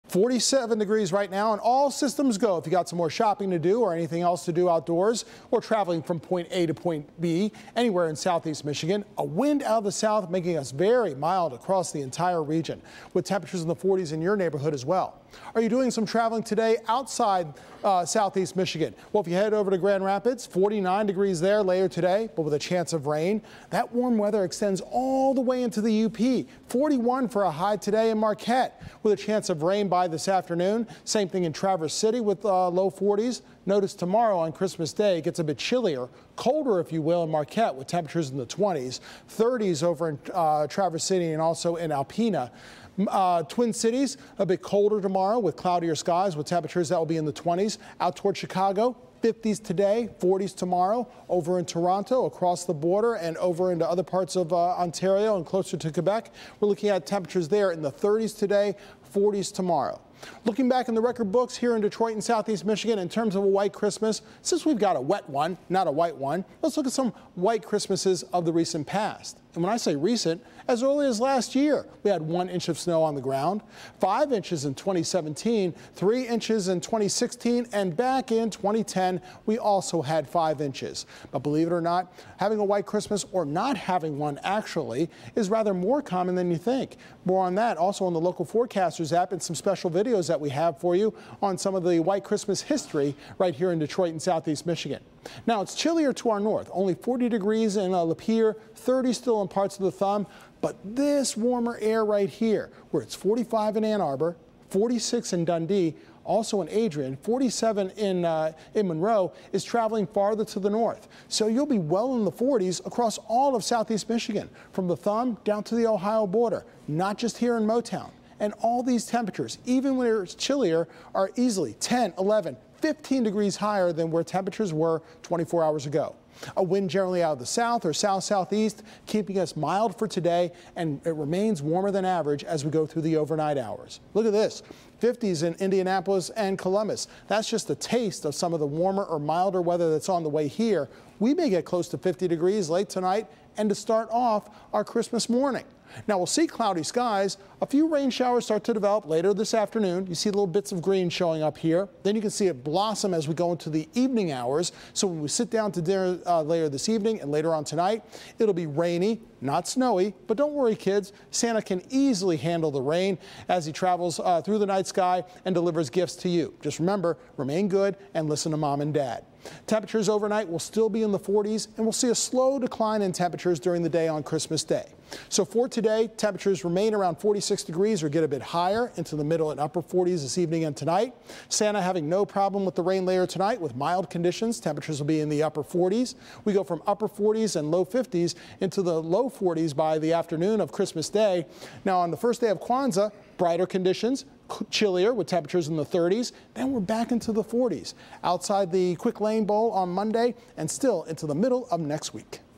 Metro Detroit weather forecast for Dec. 24, 2021 -- Noon Update
Here is the weather forecast for Metro Detroit.